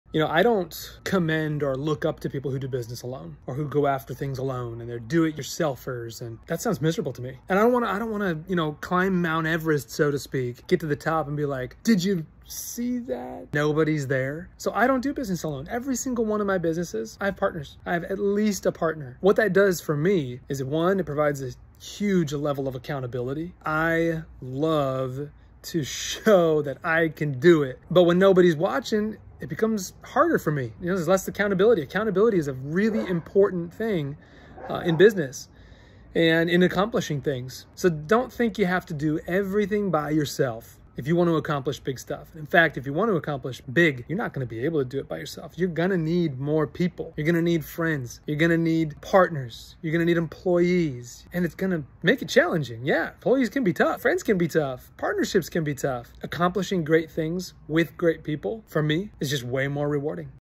Don't do it alone
Accountability is important in business and for accomplishing anything, so don't think you have to do it all yourself!